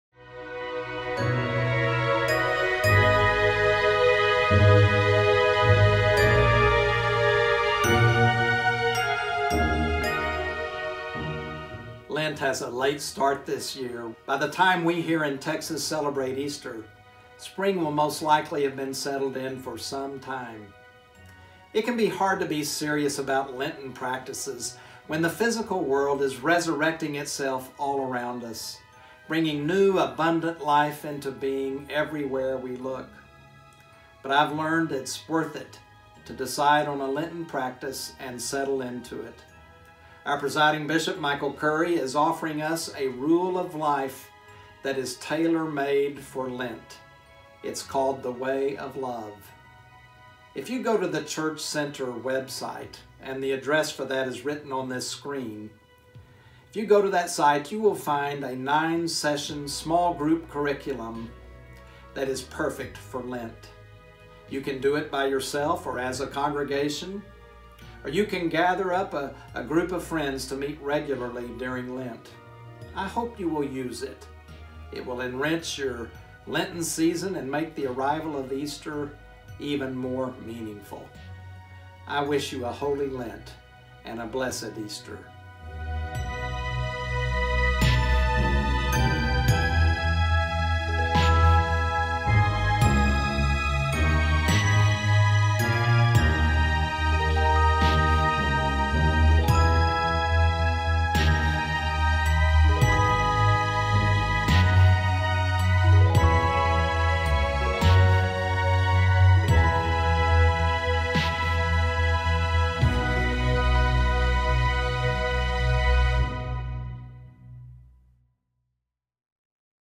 Ash Wednesday to Easter, the Way of Love
Bishop Scott Mayer invites us into a holy Lent, using a resource from the Church Center Way of Love online offerings at
Photos from Unsplash
Butterfly: Scott Carroll
Bluebonnets: Roberta Guillen
Windmill: Matthew T. Rader
Sunrise: Master Wen
Flowers in the Rain: Freedom Trail Studio used with permission